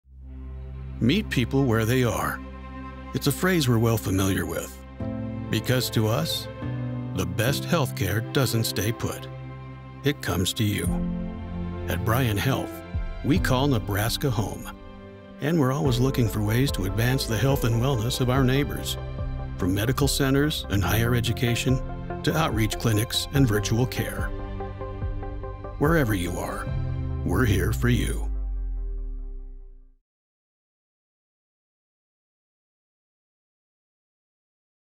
Wherever You Are, We're Here For You | Bryan Health
"Meet people where they are."

It's a phrase we're well familiar with. Because to us, the best health care doesn't stay put. It comes to you.

At Bryan Health, we call Nebraska home. And we're always looking for ways to advance the health and wellness of our neighbors. From medical centers and higher education, to outreach clinics and virtual care.

Wherever you are, we're here for you.